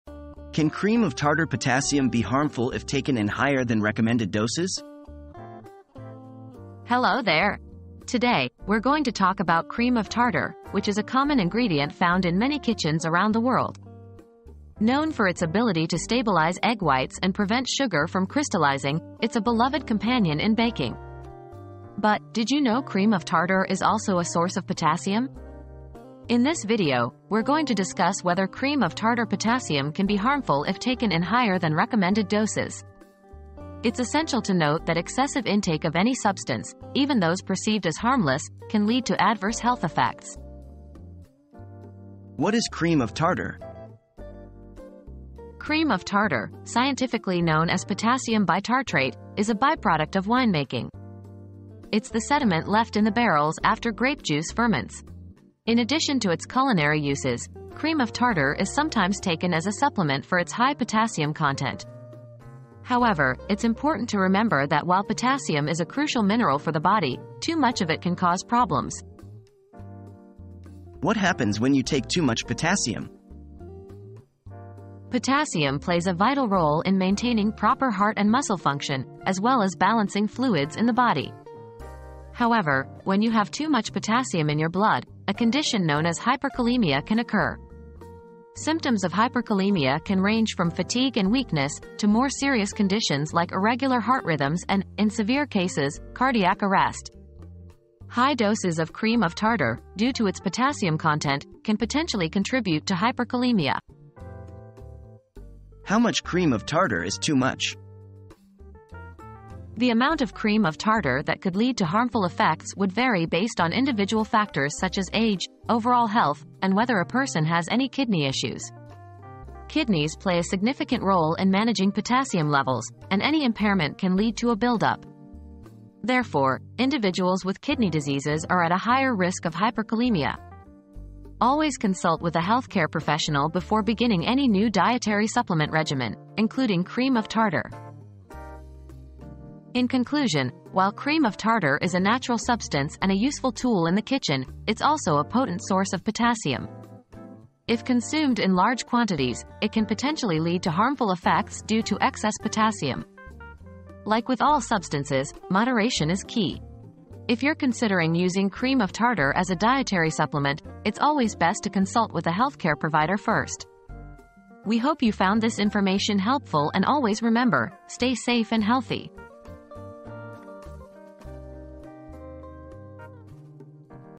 Can cream of tartar potassium be harmful if taken in higher than recommended doses?
The Impact of High Doses of Cream of Tartar Potassium on Health • Is cream of tartar potassium safe? Find out in this eye-opening video! Learn about the potential health risks of taking high doses of cream of tartar, a common ingredient found in many kitchens. Discover the impact of excessive intake of cream of tartar potassium on your health and why it's crucial to be mindful of your potassium intake. Don't miss out on this informative video!

00:00 • Can cream of tartar potassium be harmful if taken in higher than recommended doses?
00:42 • What is Cream of Tartar?
01:10 • What Happens When You Take Too Much Potassium?
01:45 • How Much Cream of Tartar is Too Much?